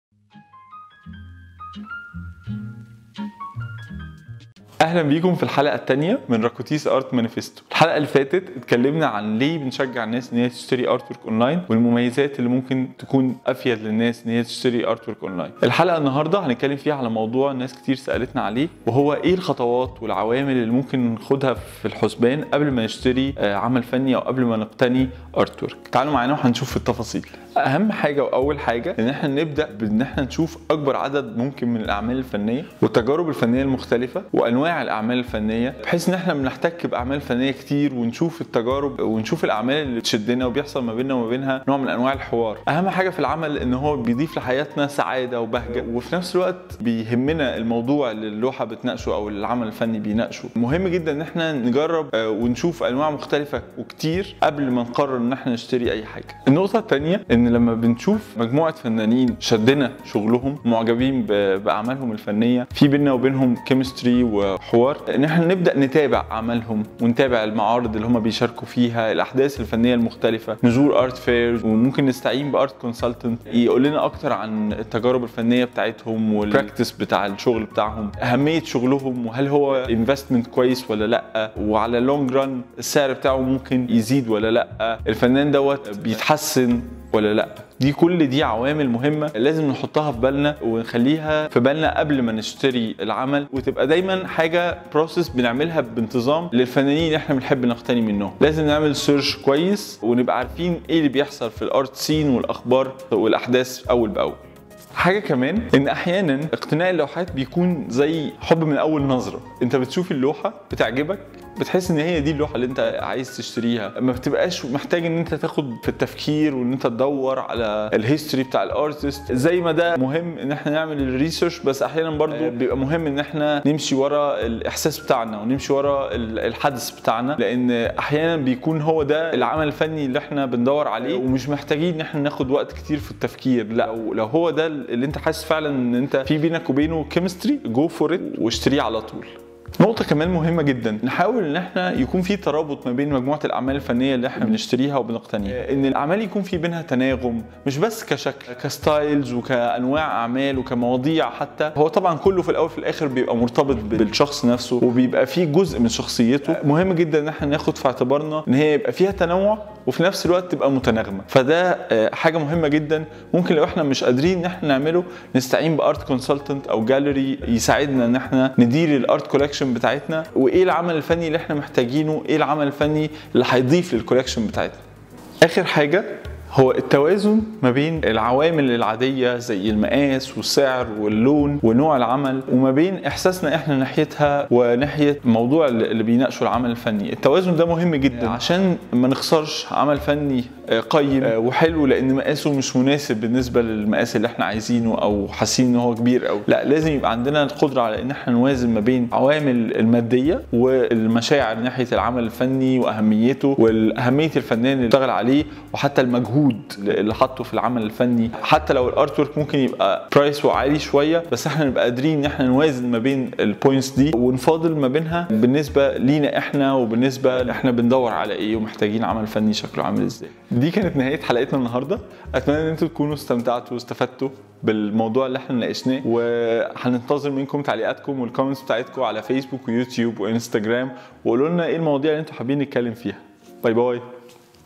"What are the steps and factors to take into account before owning Artwork?"
It's one of the highly requested topics and that's why we are tackling this one in our 2nd Episode of Racotis Art | Manifesto.

Feel free to recommend topics for our next episodes.